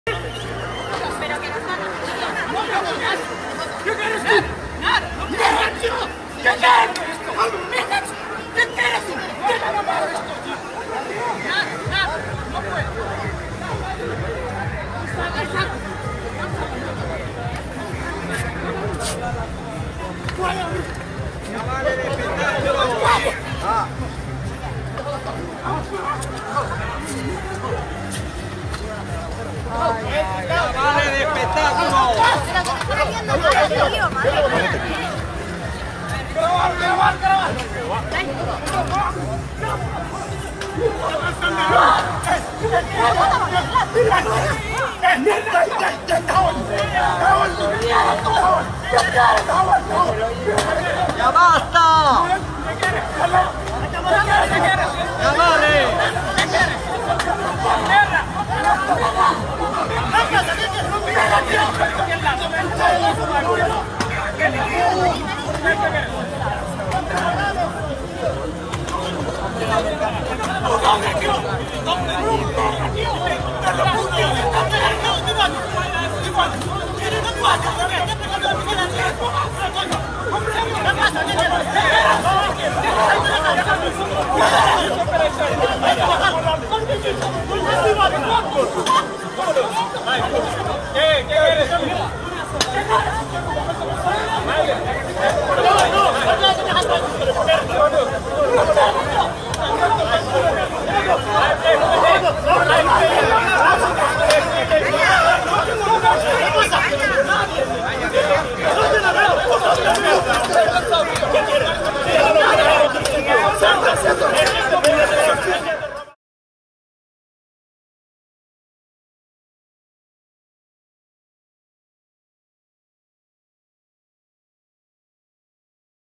Tensión entre Mossos d'Esquadra y manteros en Segur de Calafell
Un grupo de manteros se encaró con los Mossos d'Esquadra la noche del viernes 9 de agosto de 2019 en la localidad tarraconense de Segur de Calafell. La policía autonómica
trataba de impedir la venta ilegal de productos en la zona del puerto y fue cuando el grupo de vendedores empezó a recriminar la actuación de la policía hasta el punto de intentos de agresión por parte de los manteros a algunos agentes.

Imágenes cedidas por Ricard Díaz.